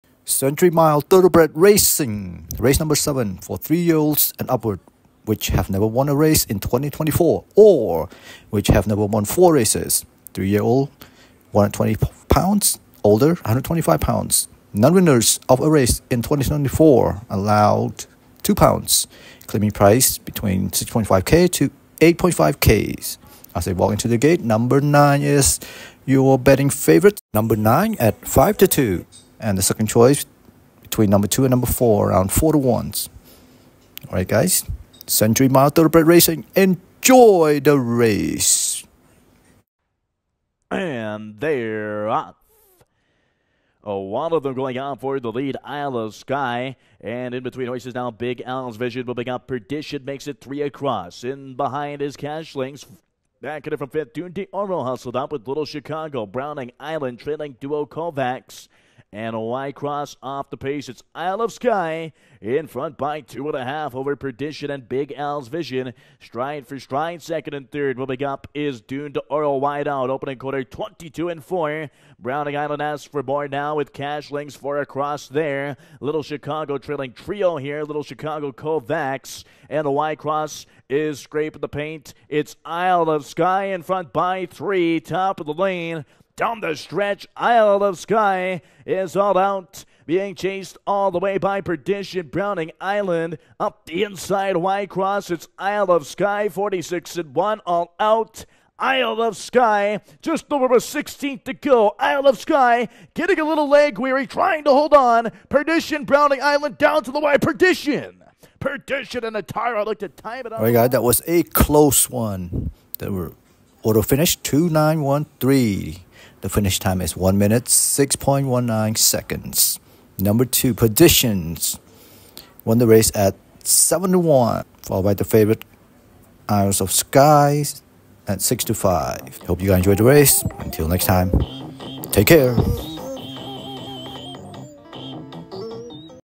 Catch me if you can
Century Mile Thoroughbred racing 2024
Claiming Race 7 - 5 1/2F purse: $14.5K

7L Horse Racing - Race Replay

Order of finish:

Perdition
Isle of Skye
Browning Island
Wye Cross
Dune d'O ro
Cashlings
Little Chicago
Kovacs
Big Al's Vision